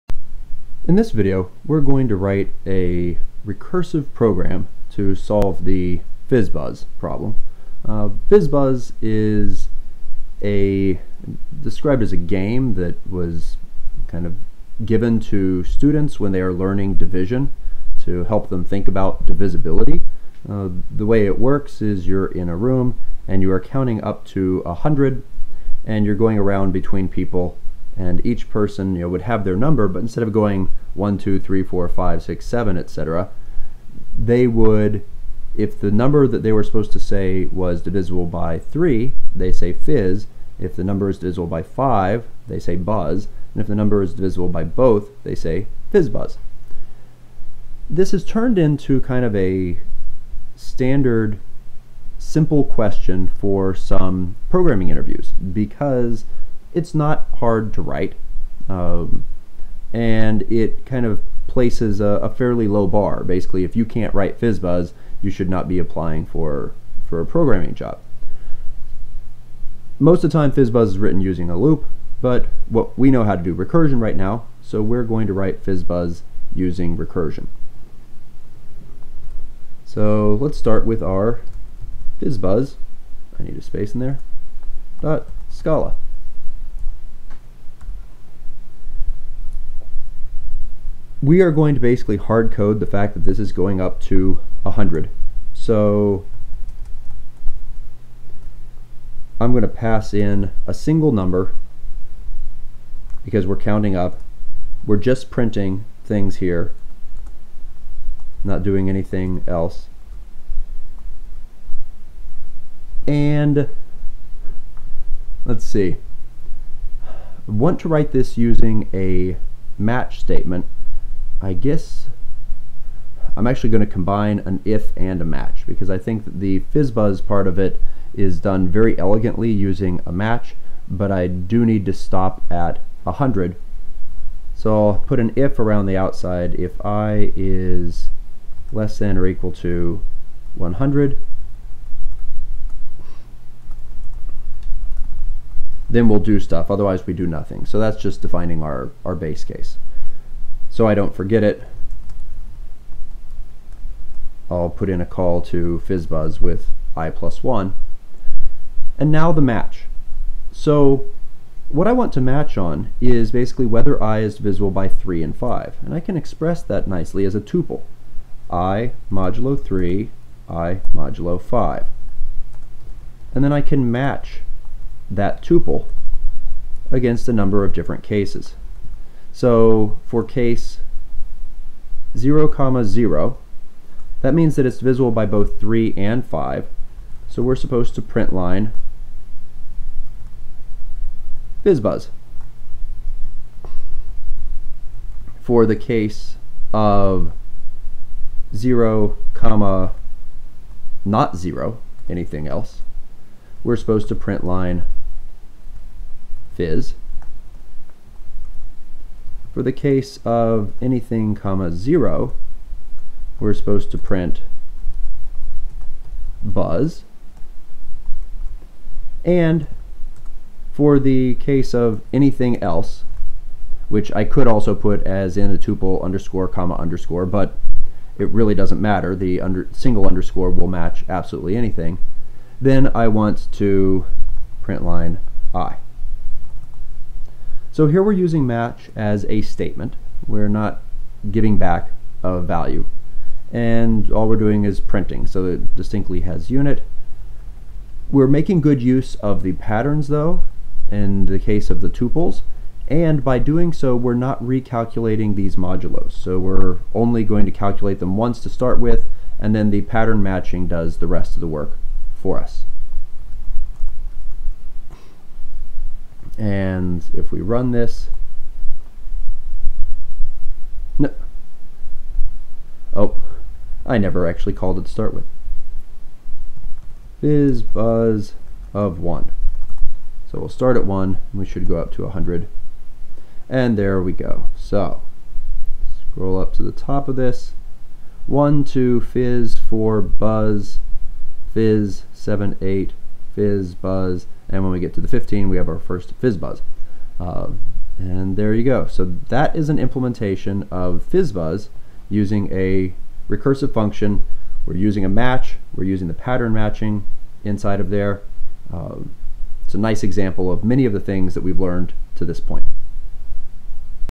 Recursive Fizz Buzz (using Scala)
This video walks through writing the classic Fizz Buzz program using recursion with a match statement.